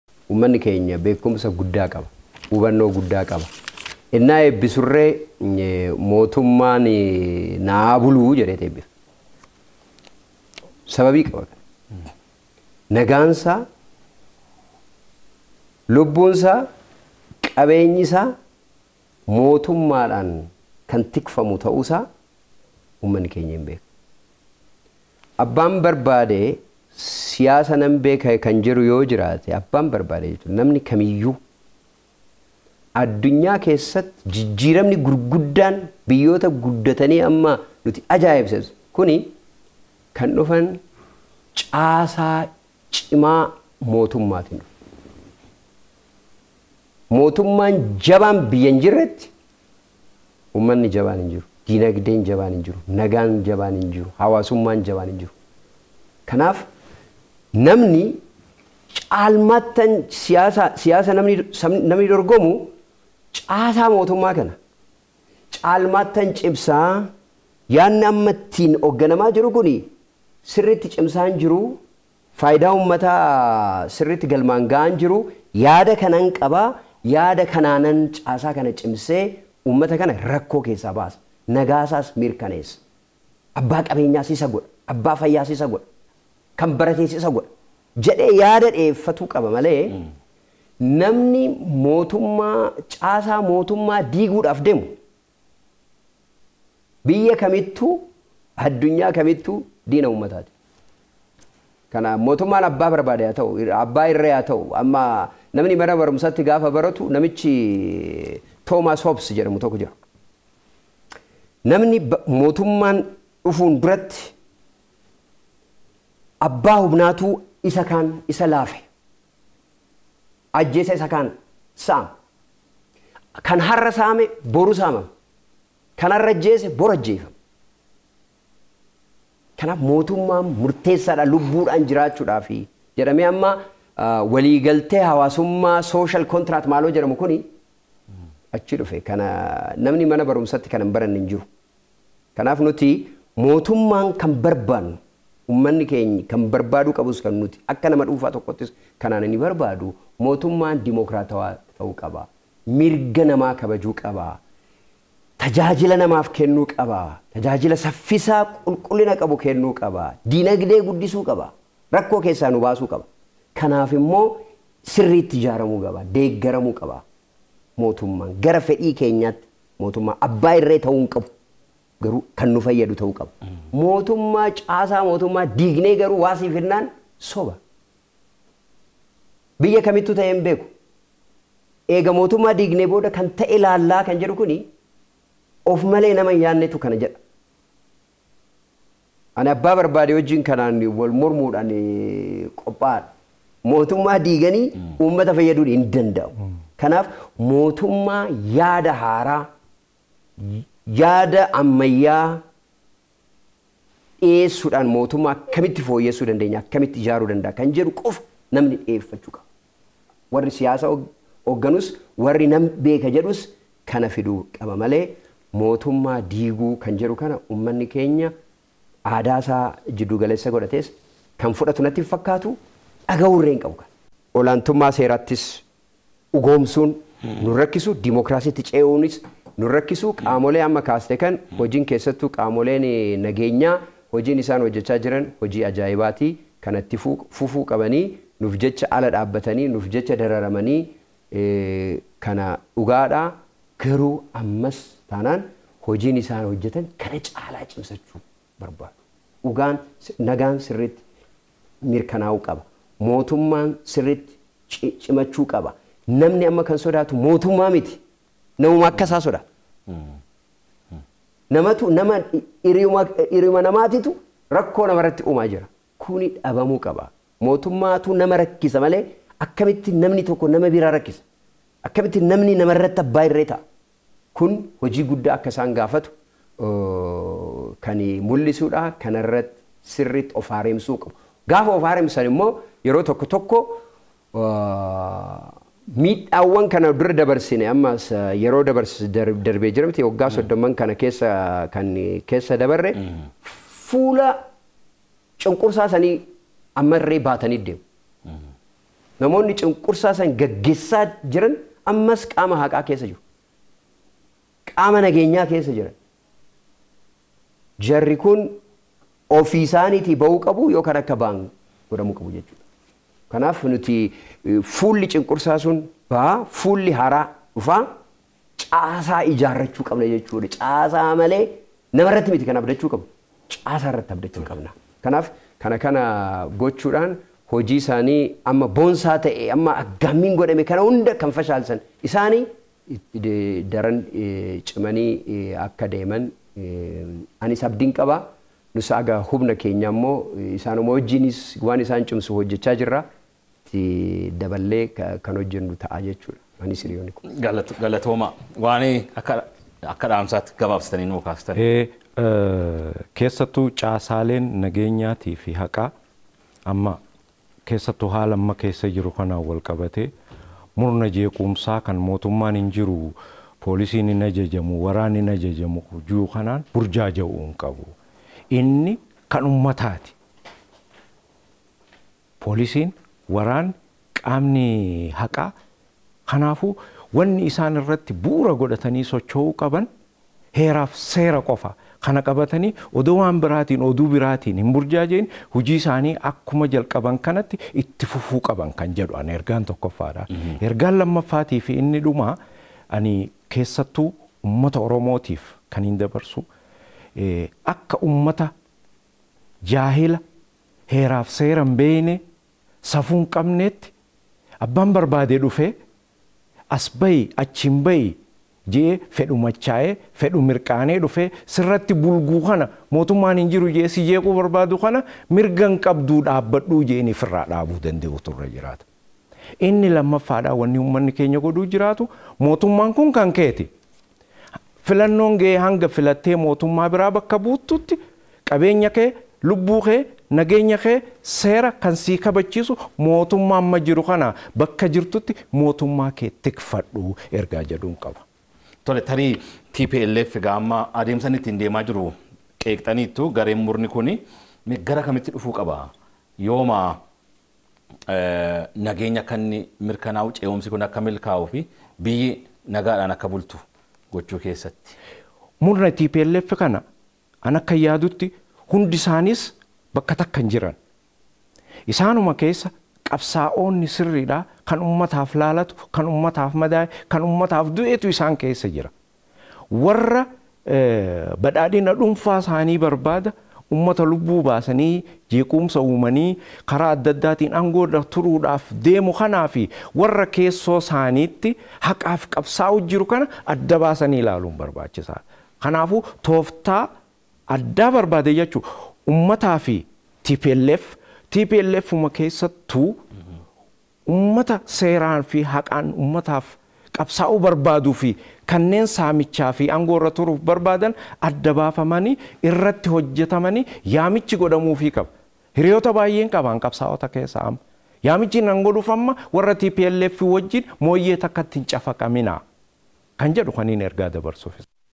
TPLF waliin mooyyee takkatti hin cafaqaminaa-Hayyoota 28 01 2013
Haala yeroo irratti turti hayyoonni OBN waliin taasisan